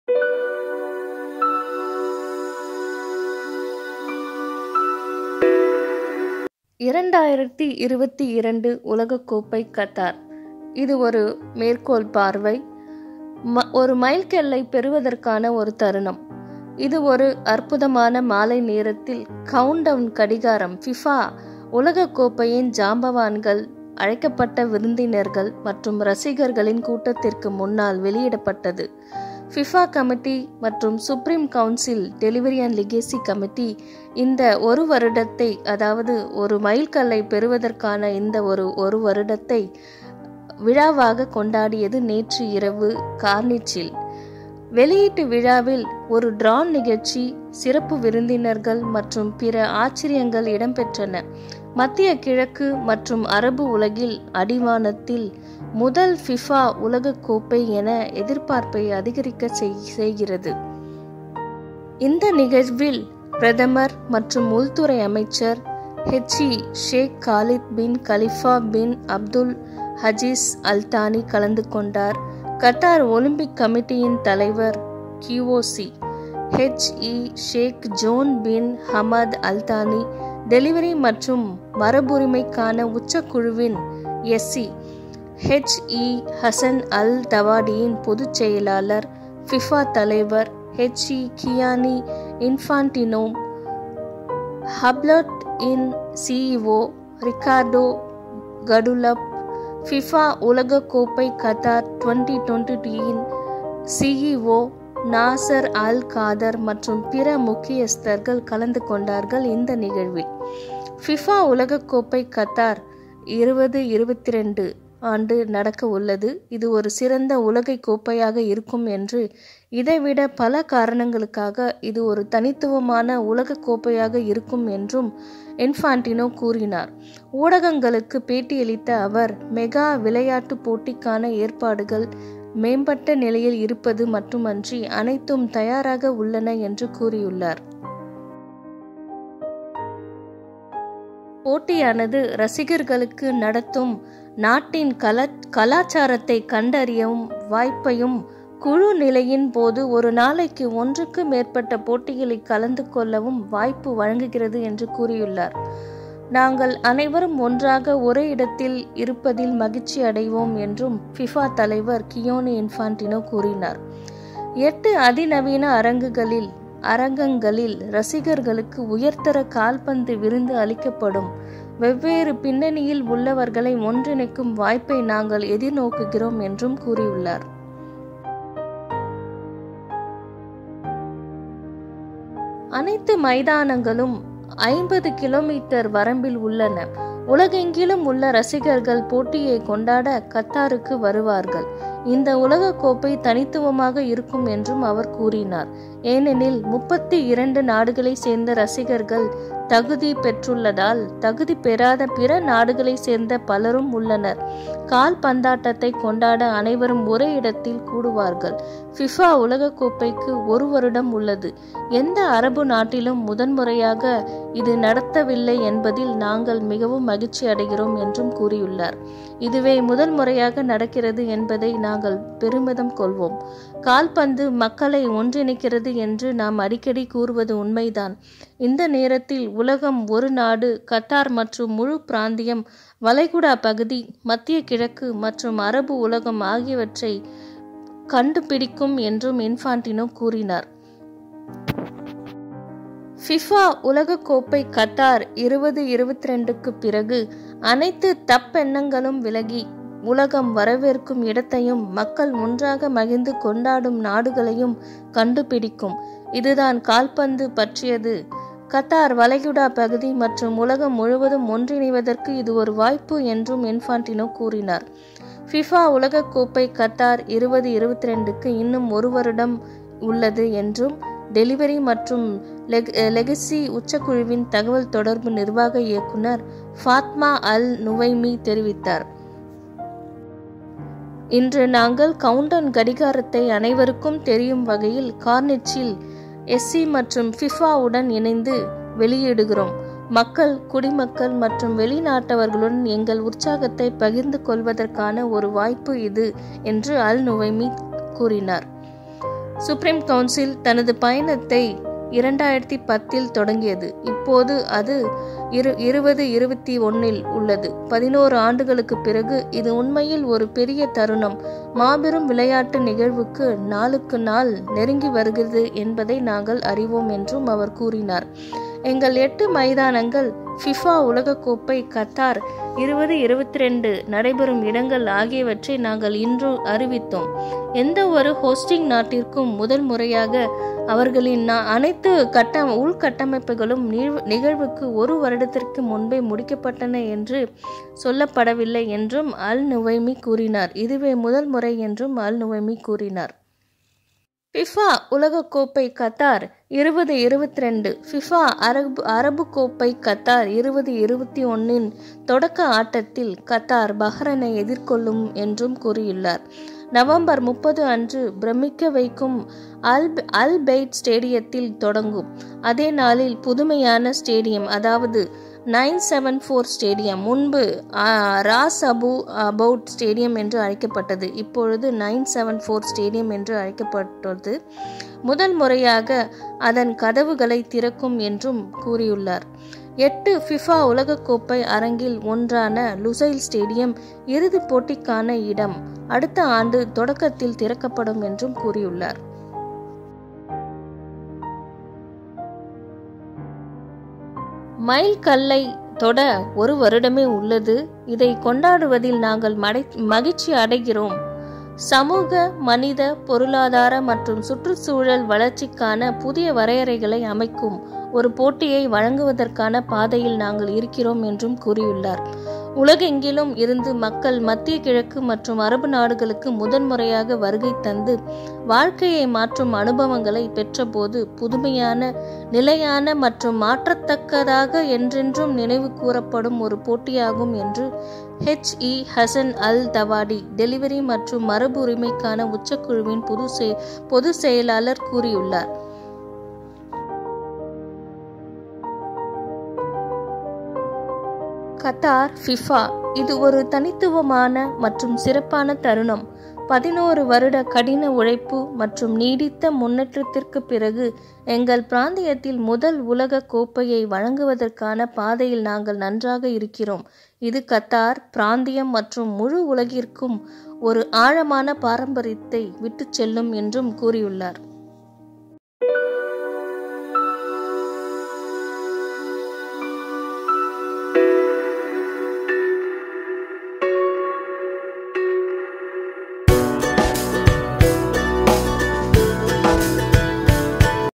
QATAR FIFA Countdown Clock l Qatar கவுண்டவுன் கடிகாரம் FIFA உலகக் கோப்பை
கவுண்டவுன் கடிகாரம் FIFA உலகக் கோப்பையின் ஜாம்பவான்கள், அழைக்கப்பட்ட விருந்தினர்கள் மற்றும் ரசிகர்களின் கூட்டத்திற்கு முன்னால் வெளியிடப்பட்டது.